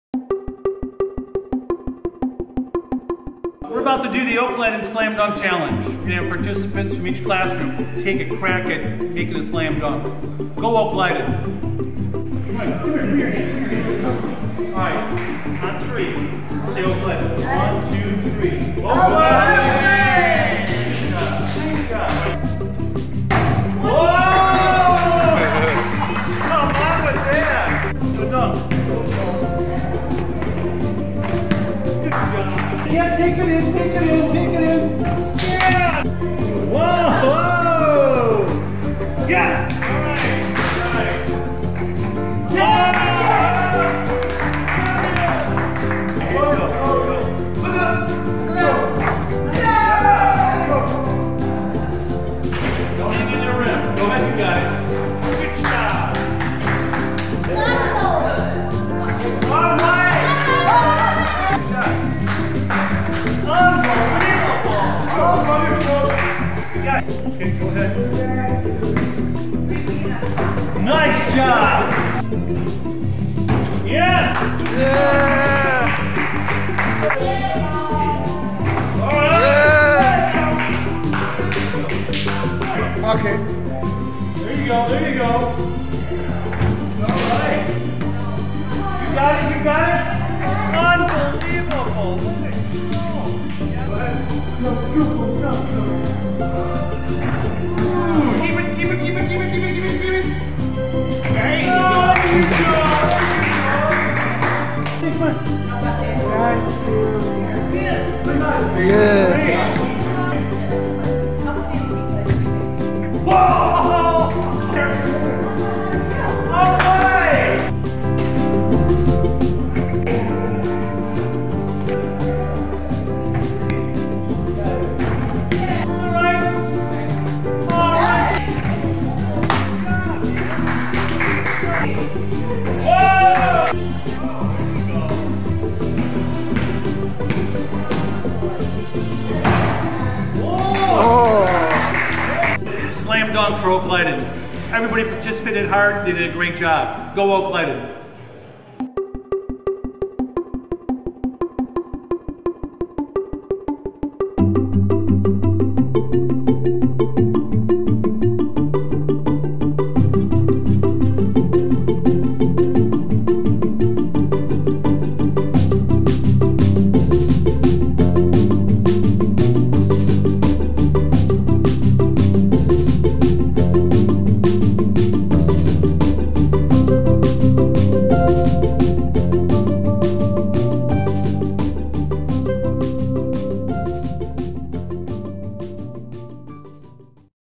Oak-Leyden Slam Dunk Challenge - Lifelong Learning Participants Dunk!
Are you fearless enough to show you care? Take the Slam Dunk Challenge in support of National Developmental Disabilities Awareness Month!

1) ACCEPT the challenge during the month of March.

2) RECORD a video of yourself slam dunking (be creative) to increase awareness of developmental disabilities.

3) UPLOAD your video to social media, tagging, or challenging at least three of your friends/family members.

4) Make a donation to support life-changing programs for children and adults with developmental disabilities in your community